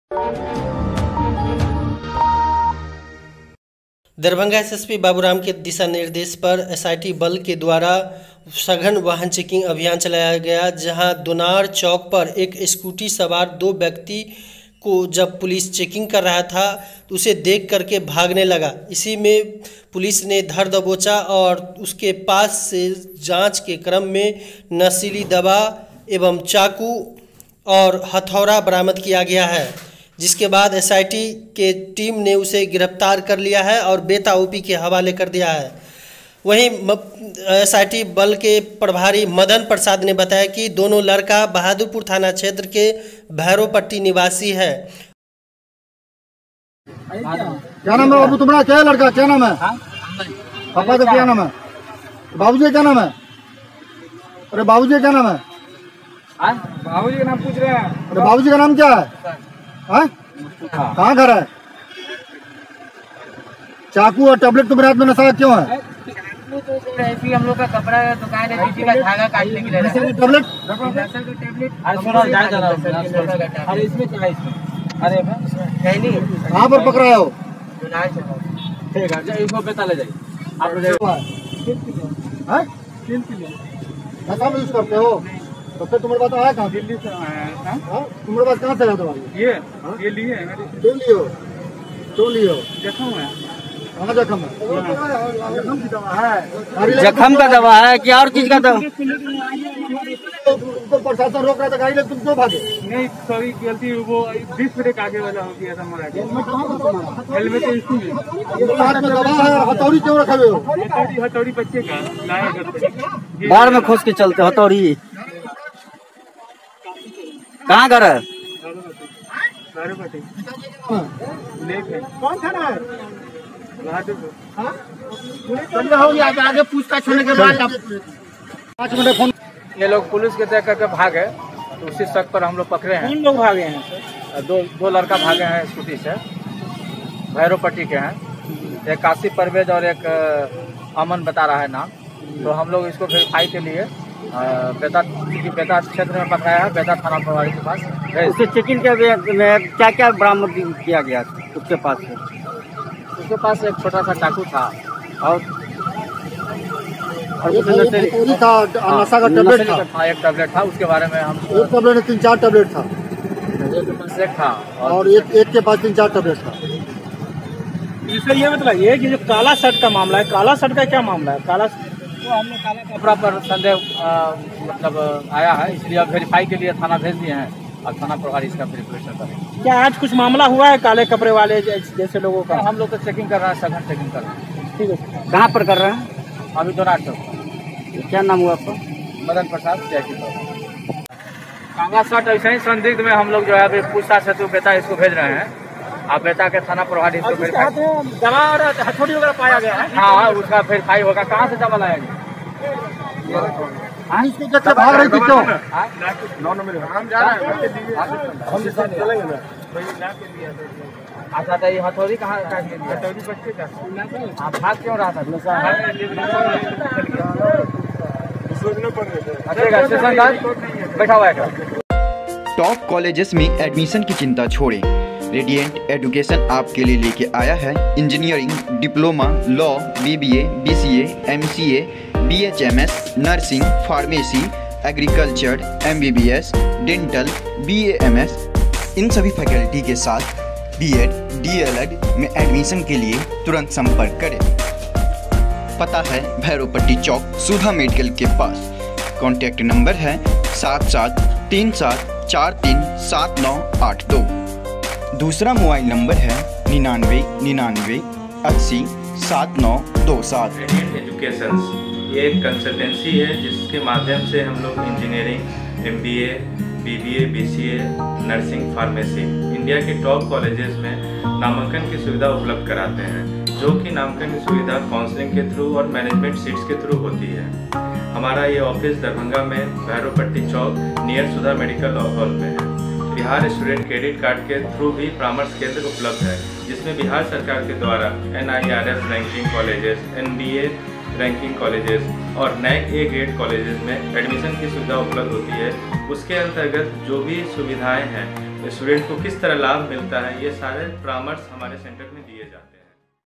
दरभंगा पुलिस को मिली कामयाबी, दोनार चौक से नशीली दवा चाकू हथौरा के साथ दो युवक गिरफ्तार
वरीय पुलिस अधीक्षक के दिशा निर्देश पर सीआईटी के जवानों के द्वारा सघन वाहन चेकिंग अभियान चलाया गया जहां दोनार चौक पर एक स्कूटी सवार दो व्यक्ति को जब पुलिस चेकिंग कर रहा था देखकर भागने लगा उसे पकड़ा कर जांच किया तो उसके पास से नशीली दवा एवं चाकू एक छोटा सा हथोड़ा भी बरामद किया
सीआईटी के प्रभारी मदन प्रसाद ने बताया कि दोनों लड़का बहादुरपुर थाना क्षेत्र के भैरोपट्टी निवासी है